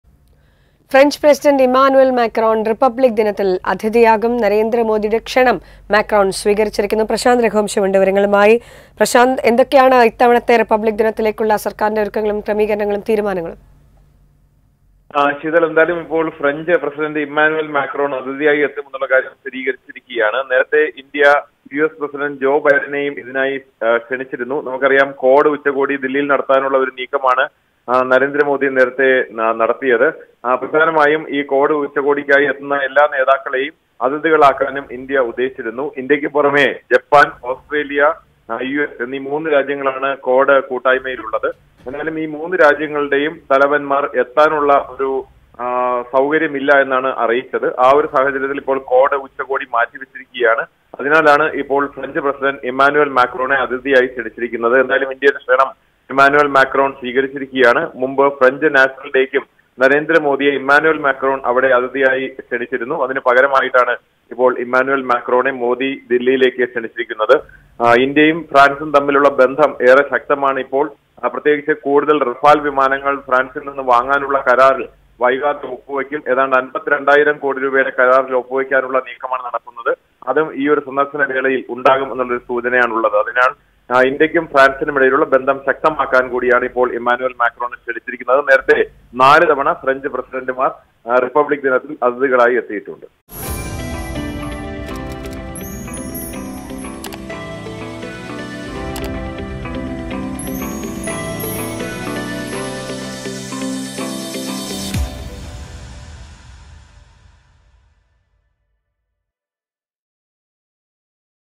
ഫ്രഞ്ച് പ്രസിഡന്റ് ഇമ്മാനുവൽ മാക്രോൺ റിപ്പബ്ലിക് ദിനാഘോഷത്തിൽ അതിഥിയാകും | Emmanuel Macron
നരേന്ദ്രമോദിയുടെ ക്ഷണം സ്വീകരിച്ച് ഫ്രഞ്ച് പ്രസിഡന്റ്; ഇമ്മാനുവൽ മാക്രോൺ അടുത്ത റിപ്പബ്ലിക് ദിനാഘോഷത്തിൽ അതിഥിയാകും
Republic Day 2024 | French President | Emmanuel Macron